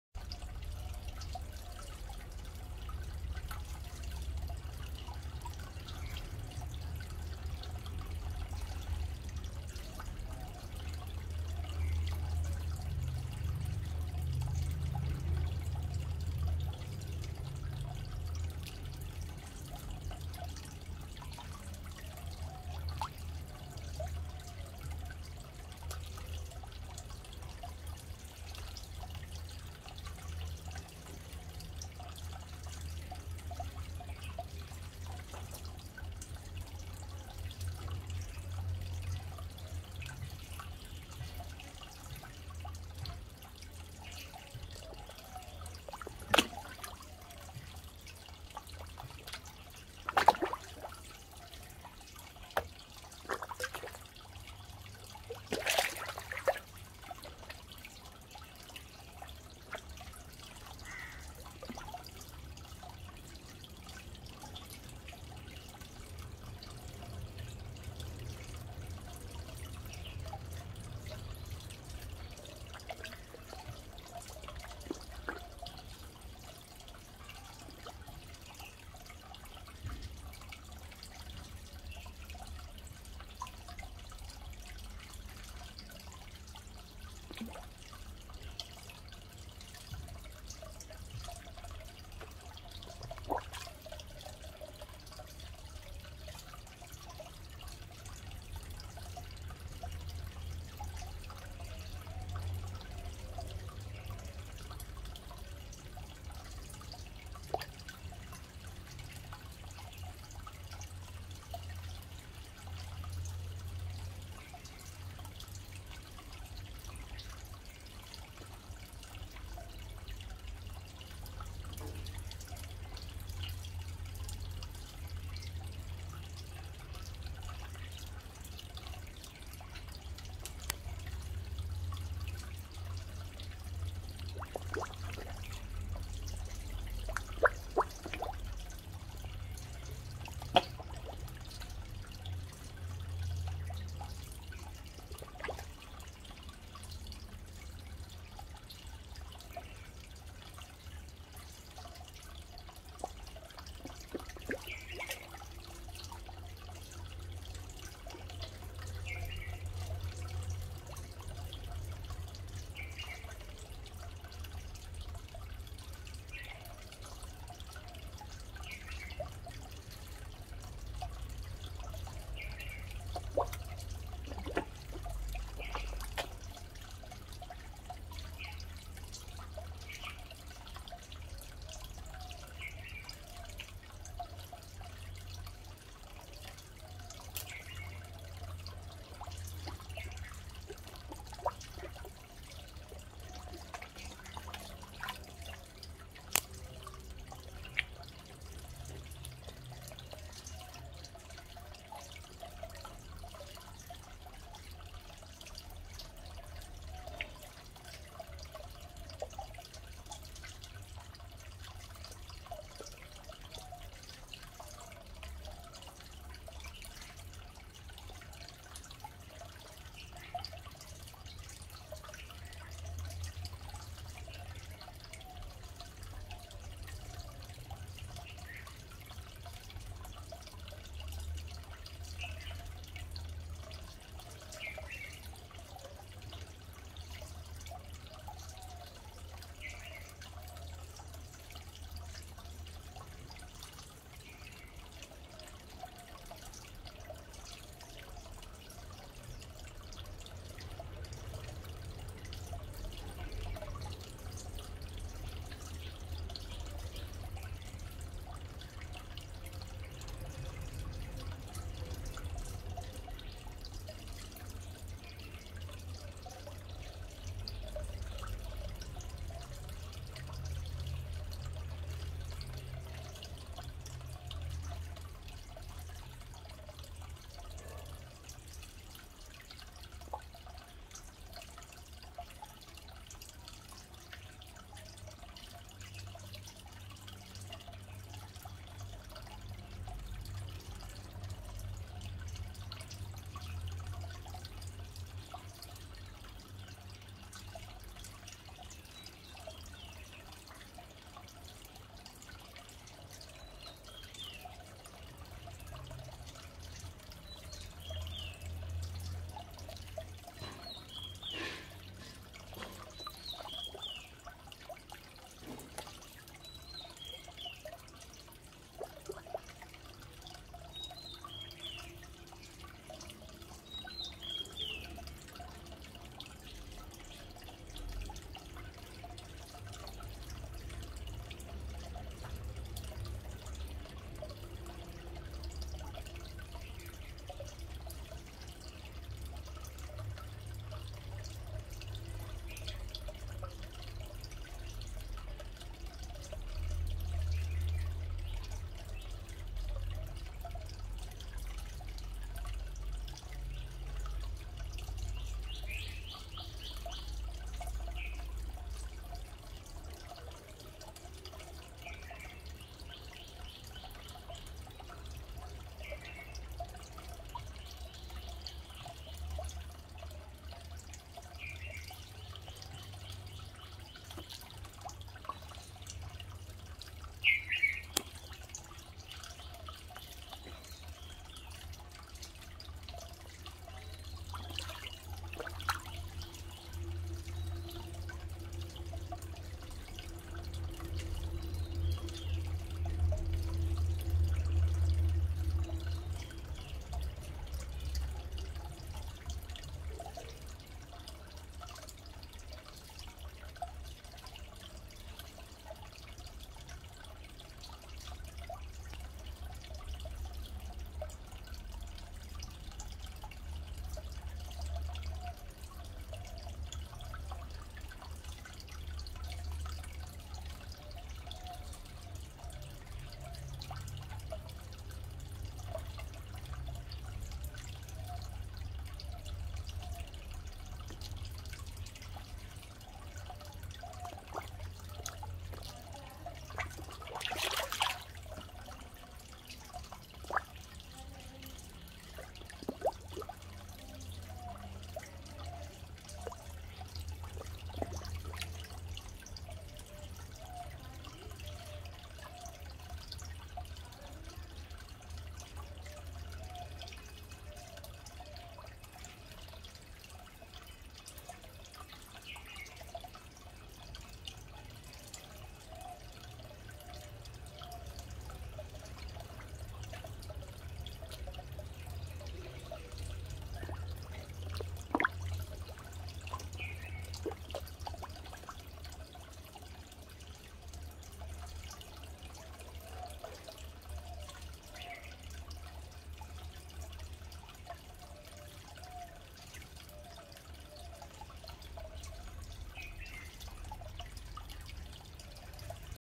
KOI FEEDING THERAPY IN NATURAL POND | RELAXING SOUND
KOi feeding in natural pond in summer. Enjoy this relaxing video of me feeding my pond.